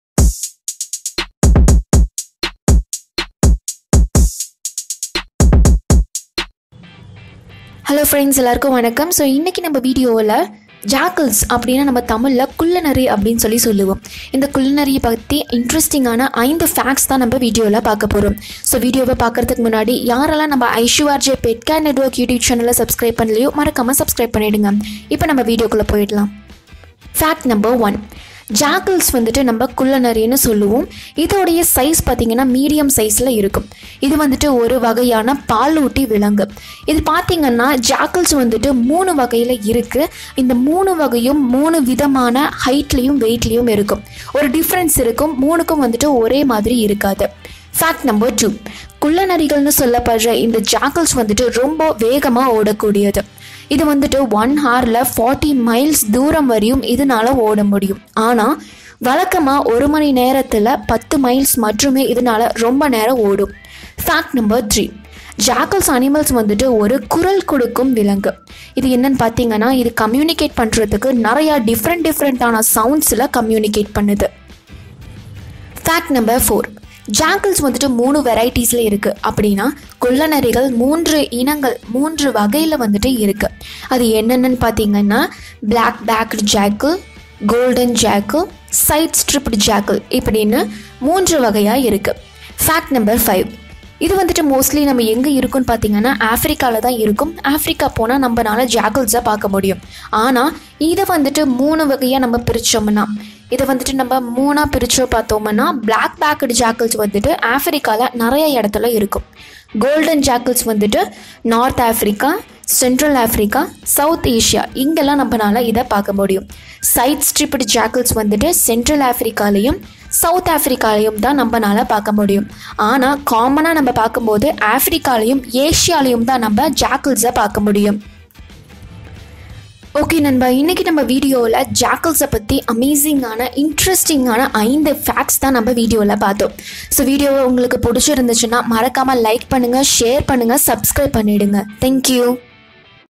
குள்ளநரிகளைப் பற்றிய 5 சுவாரஸ்யமான உண்மைகள் | Tamil | 5 Interesting facts about jackal
This video say about 5 amazing and interesting facts about jackal animal facts in Tamil.